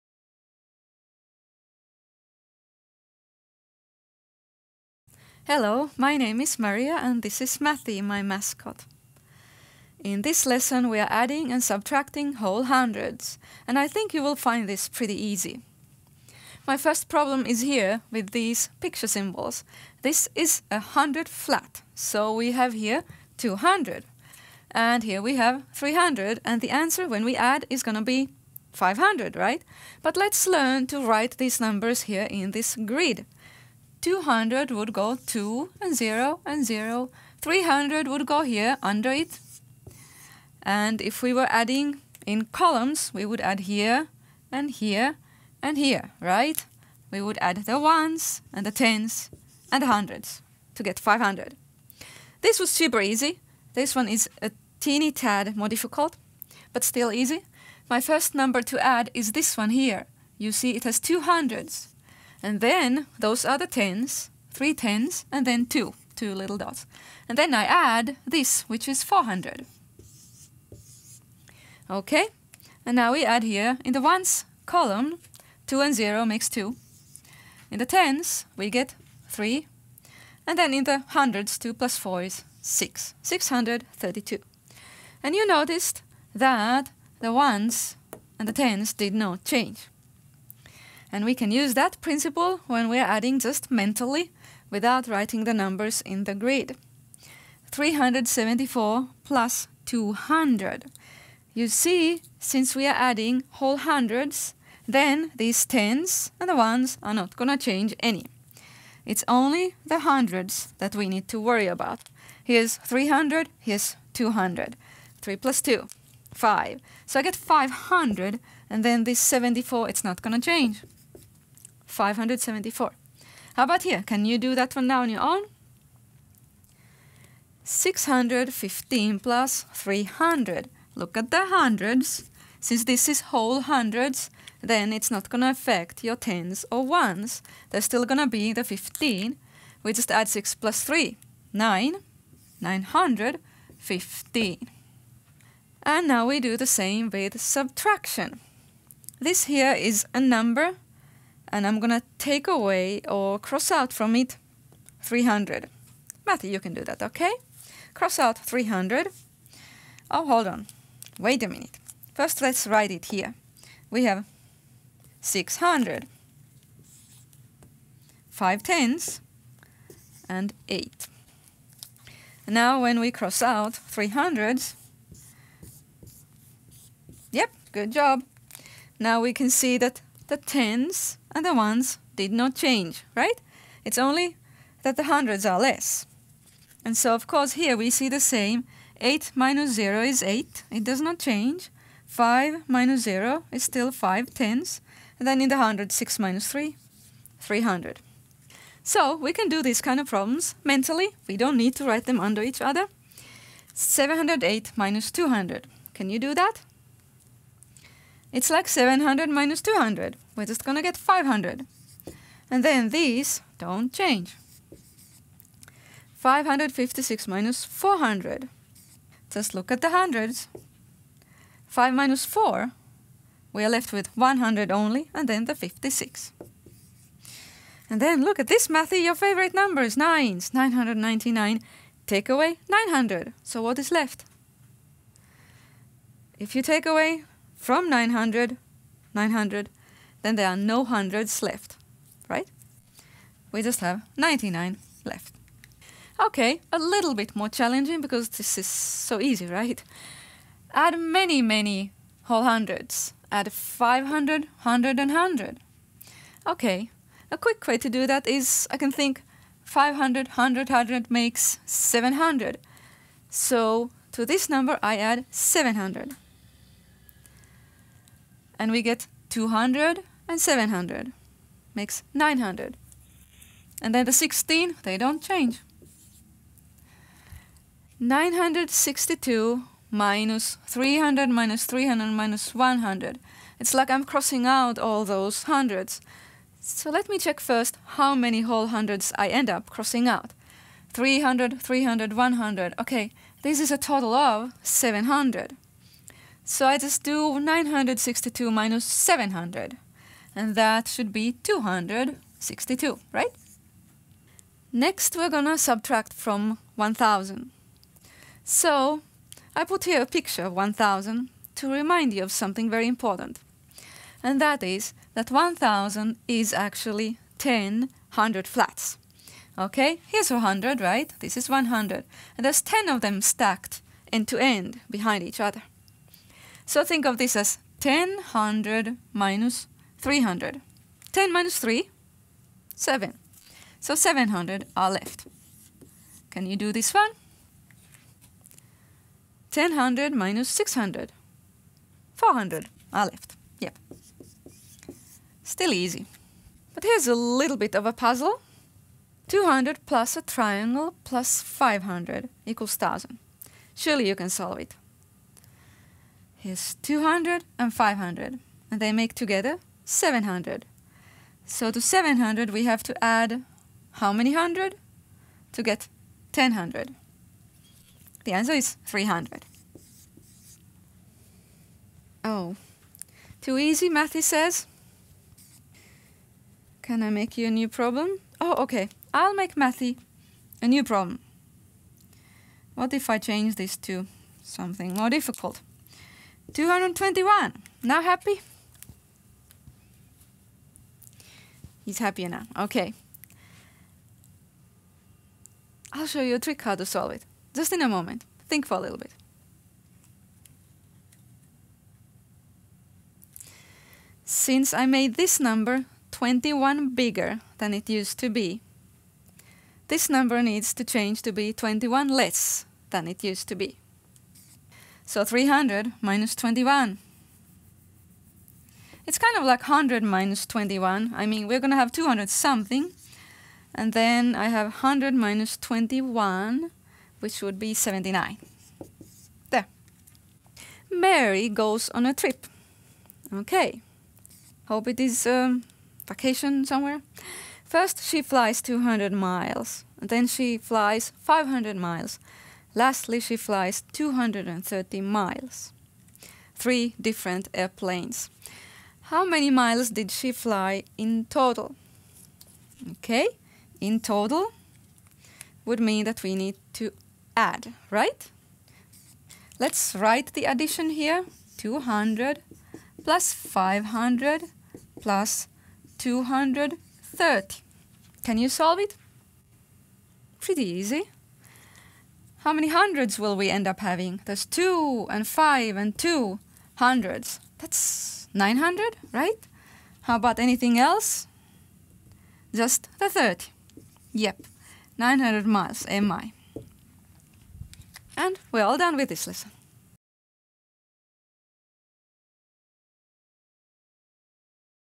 Add and subtract multiples of 100 - mental math
In this mental math lesson, I show how easy it is to add and subtract whole hundreds (numbers such as 200, 500, 600, etc.) to any other 3-digit number. It really is so easy that Mathy, my mascot, at one point asks for a more difficult problem - so I provide one for him. :)

Of course, adding and subtracting multiples of 100 mentally is based on place value... we only need to "worry about" the hundreds digit changing in the result.

This lesson is meant for 2nd or 3rd grade math.